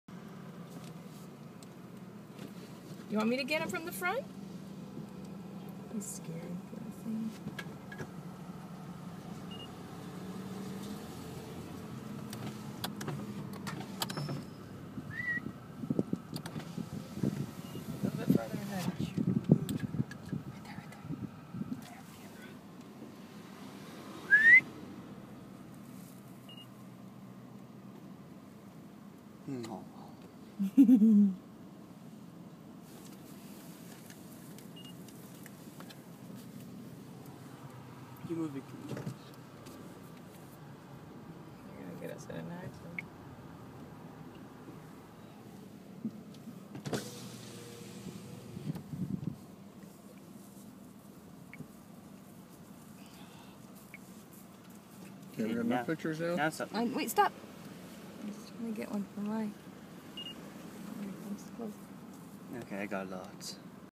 Bear in Banff Park
This bear was taking a stroll down HWY 1A